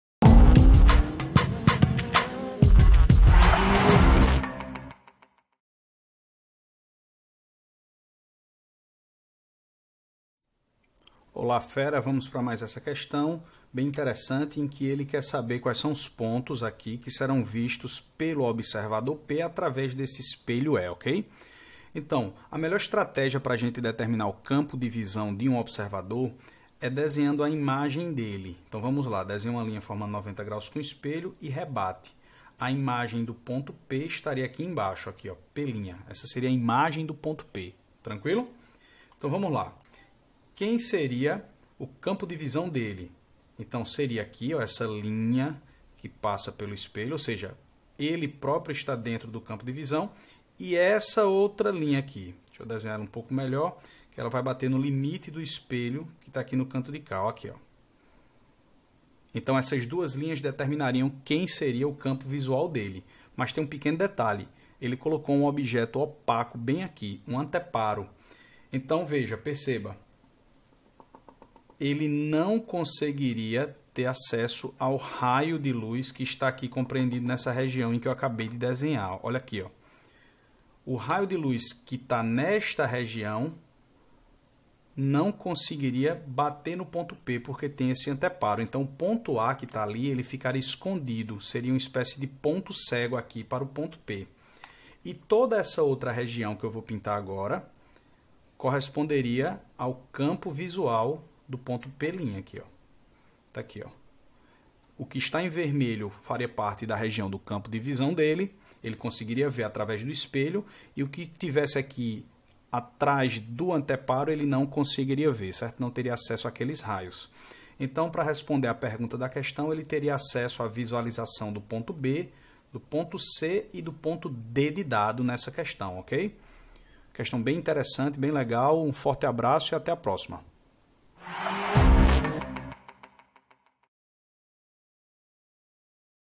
28 - Reflexão da Luz e Espelhos Planos
(PUC-RJ) Quais dos objetos A, B, C, D e E são vistos pelo
observador P ao olhar para o espelho plano
esquematizado?
A, B, C, D e E
A, E e B
A, B e C
B, C e D
C, D e E